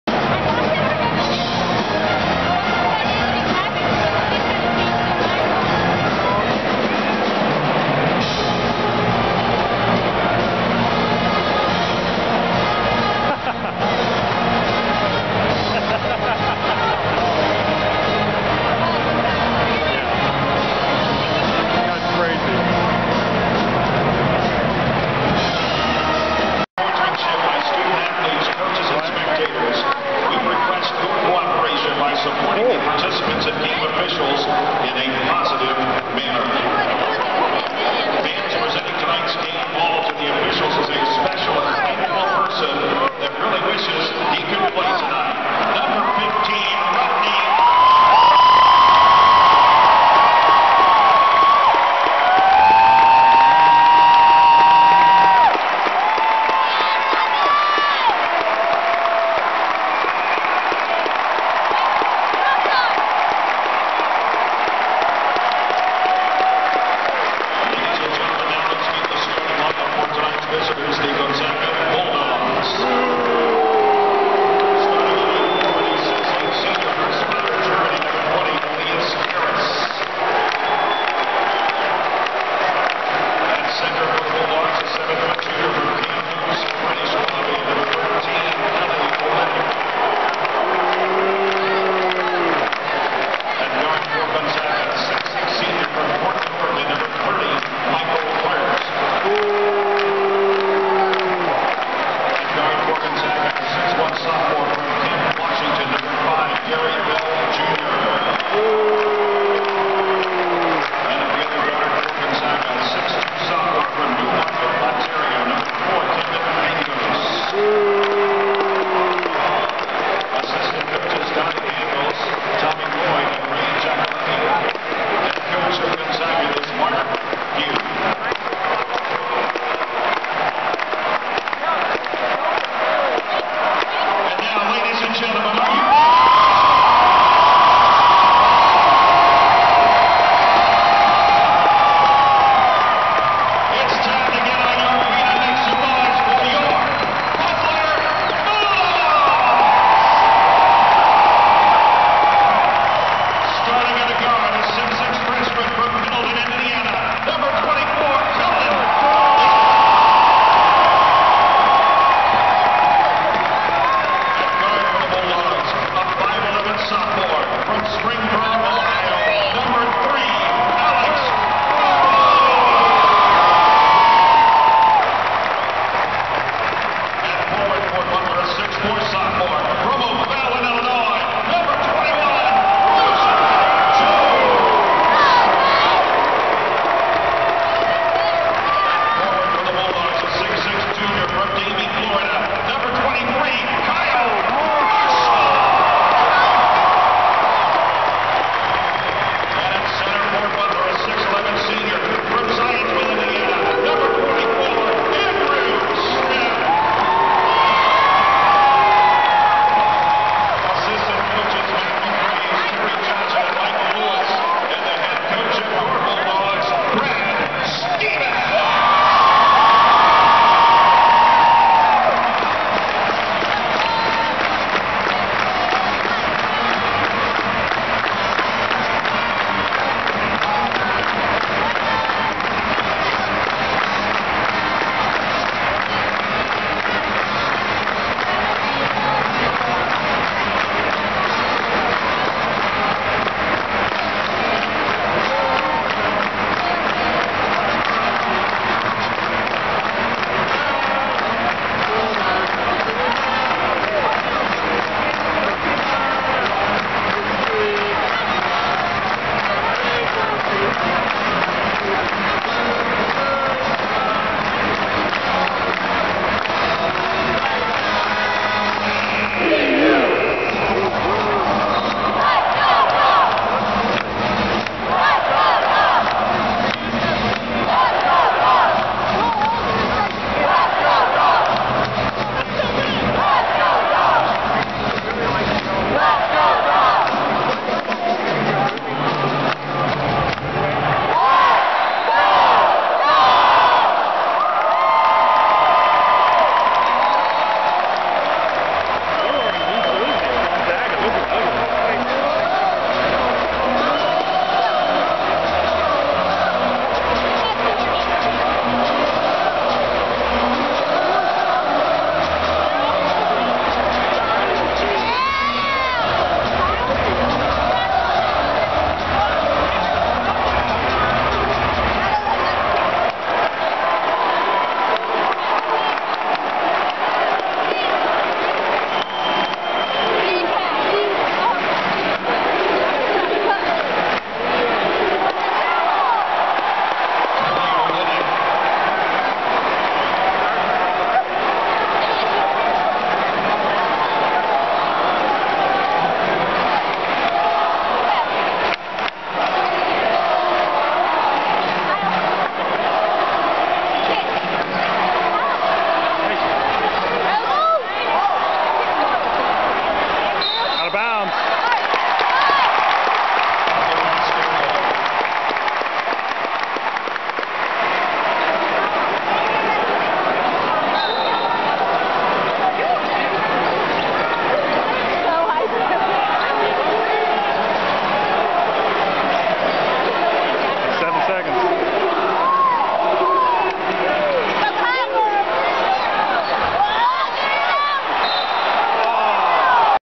Beginning of the Gonzaga at Butler Game
Video of the starting lineups and the beginning of the #8 Gonzaga Bulldogs at #13 Butler Bulldogs game. Also, a funny clip of Dickie V dancing with Hink.